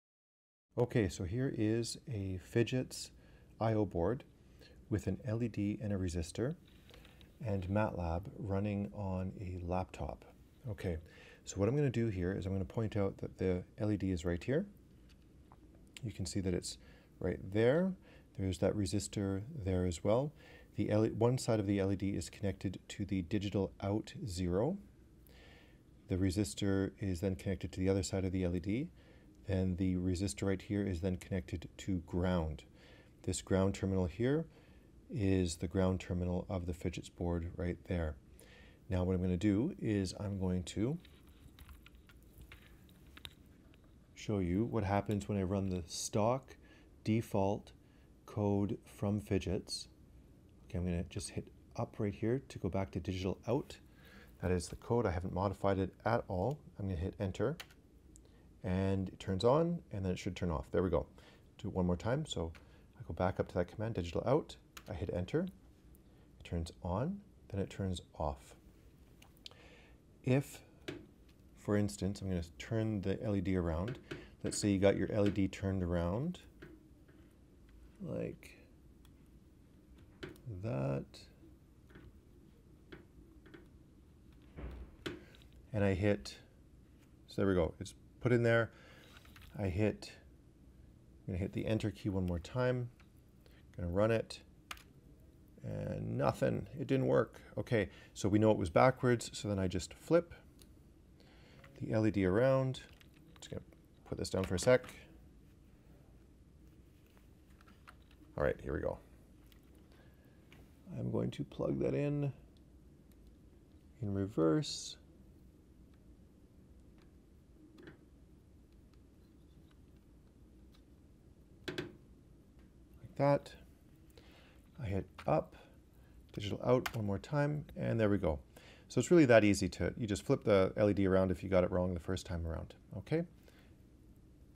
Turning on an LED with Phidgets
This is an example of using the sample Phidgets code to turn an LED on and off.